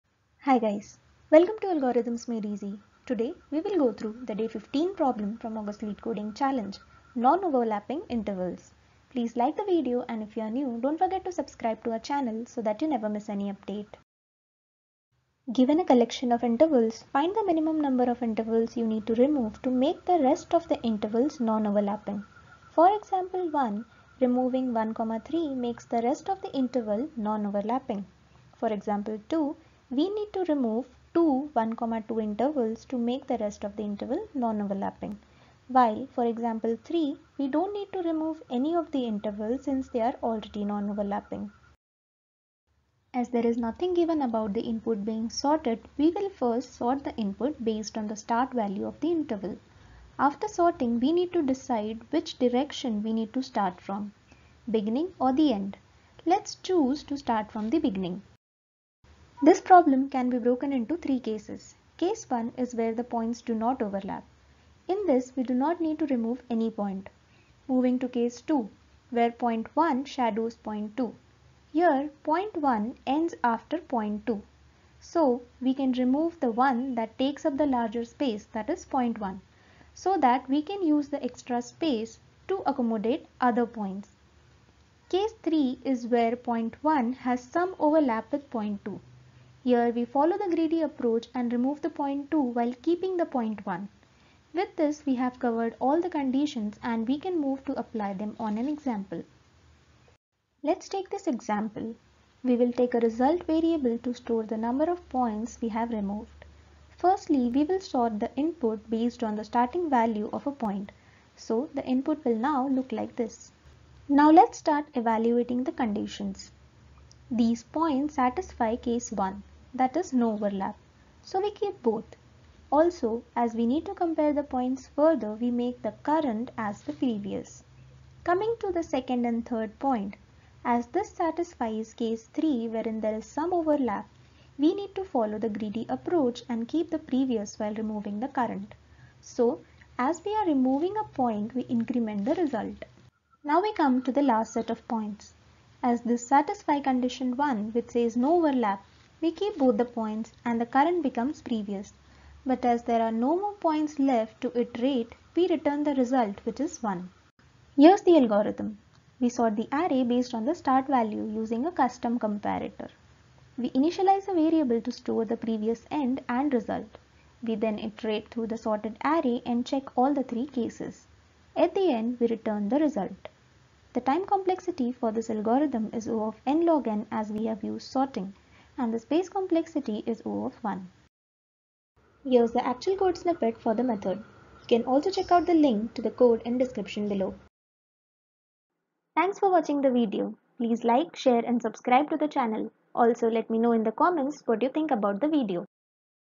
Non-overlapping Intervals | Day 15 | [ August LeetCoding Challenge ] [ Leetcode #435 ] [ 2020 ]
The day 15  problem in August Leetcoding Challenge. ( Non-overlapping Intervals ).

Problem statement:

Given a collection of intervals, find the minimum number of intervals you need to remove to make the rest of the intervals non-overlapping.

Example 1:

Input: [[1,2],[2,3],[3,4],[1,3]]
Output: 1
Explanation: [1,3] can be removed and the rest of intervals are non-overlapping.
Example 2:

Input: [[1,2],[1,2],[1,2]]
Output: 2
Explanation: You need to remove two [1,2] to make the rest of intervals non-overlapping.
Example 3:

Input: [[1,2],[2,3]]
Output: 0
Explanation: You don't need to remove any of the intervals since they're already non-overlapping.

Note:
You may assume the interval's end point is always bigger than its start point.
Intervals like [1,2] and [2,3] have borders "touching" but they don't overlap each other.